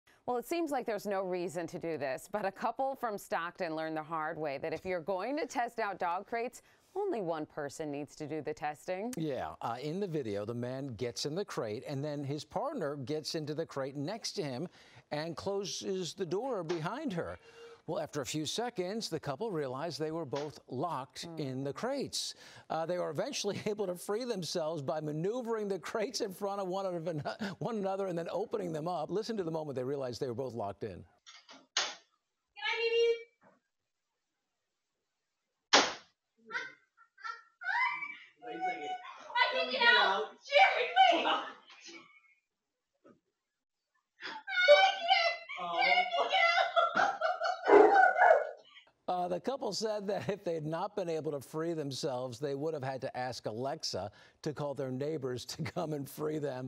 CA couple accidentally lock themselves in dog crates
A couple from Stockton learned the hard way that if you are going to test out dog crates, only one person needs to do the testing.